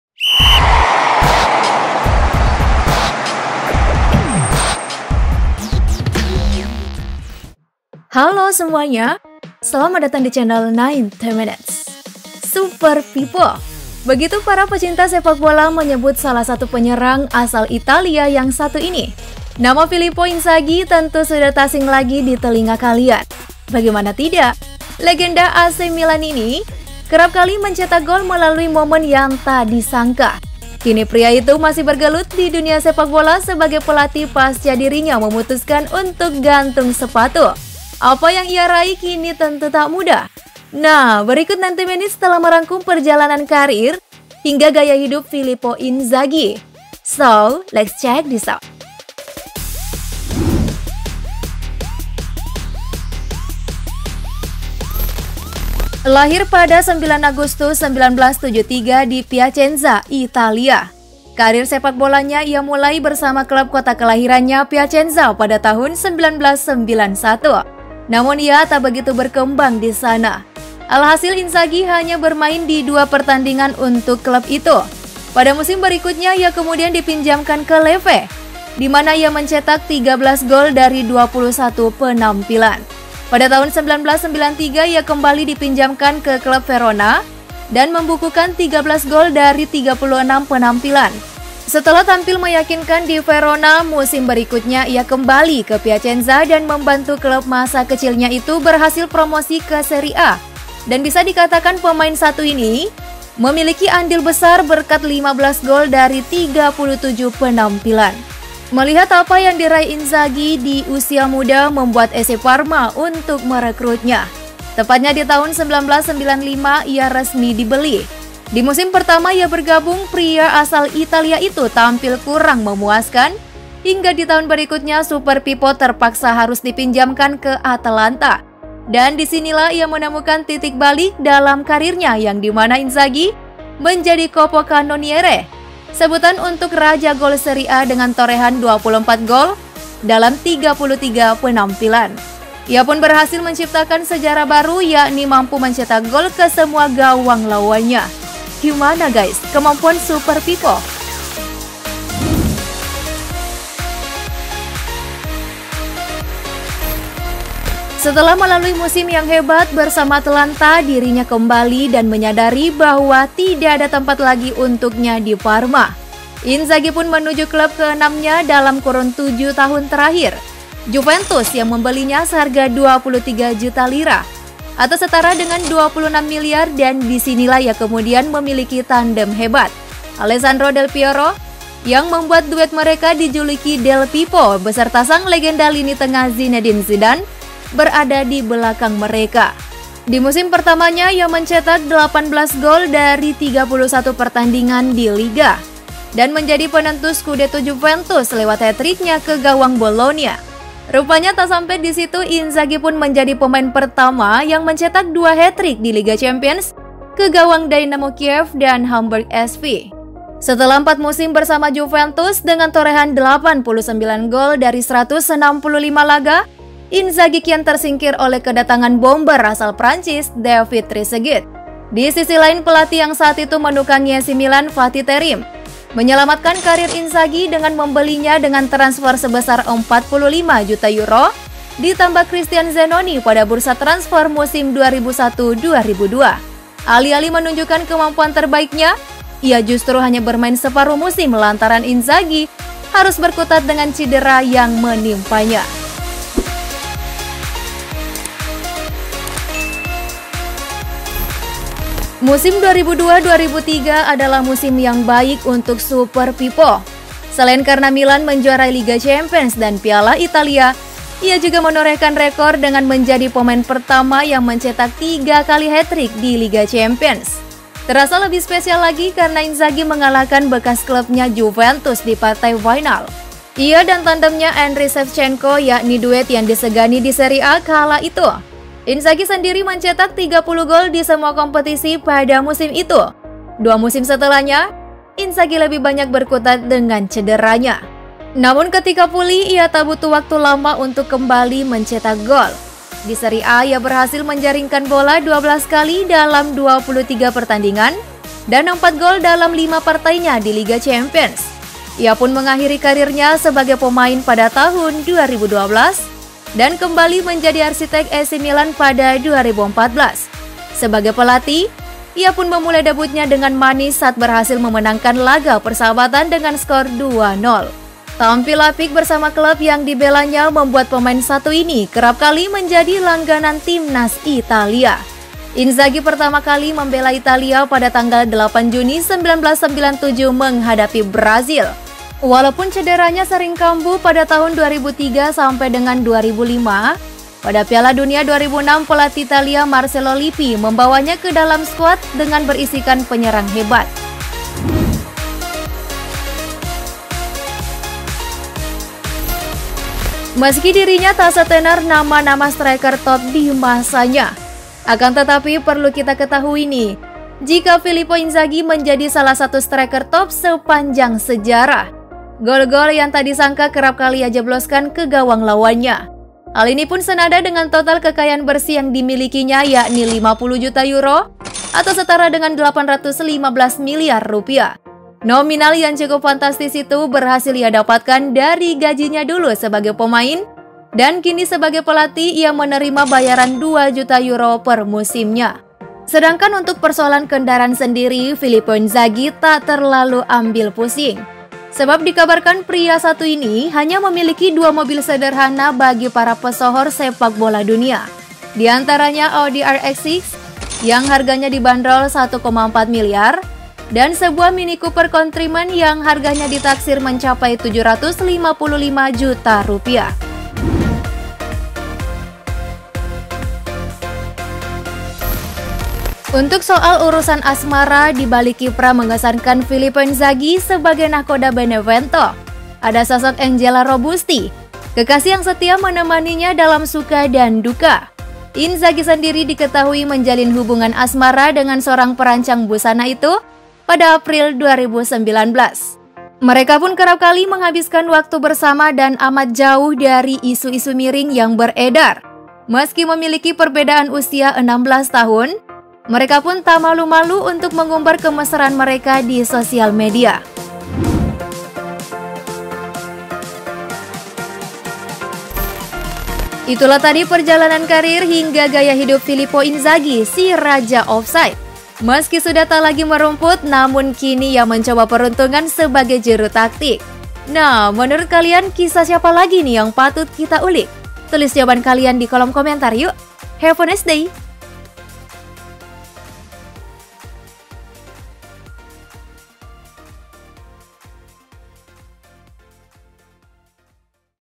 Penyerang Tajam Yg Terlahir Off-Side! Begini Perjalanan karir & gaya hidup filippo, Legenda AC MILAN
HELLO SEMUANYA
Selamat Datang di Channel 90 Minutes!!
Super Pippo, begitu para pecinta sepakbola menyebut salah satu penyerang asal Italia yang satu ini, nama Filippo Inzaghi tentu sudah tak asing lagi di telinga kalian, bagaimana tidak, legenda AC Milan ini kerap kali mencetak gol melalui momen yang tak disangkah. Kini pria itu masih bergelut di dunia sepakbola sebagai pelatih pasca dirinya memutuskan untuk gantung sepatu. Apa yang ia raih kini tentu tak mudah, nah berikut 90 minutes telah merangkum perjalanan karir hingga gaya hidup Filippo Inzhagi, So lets check this out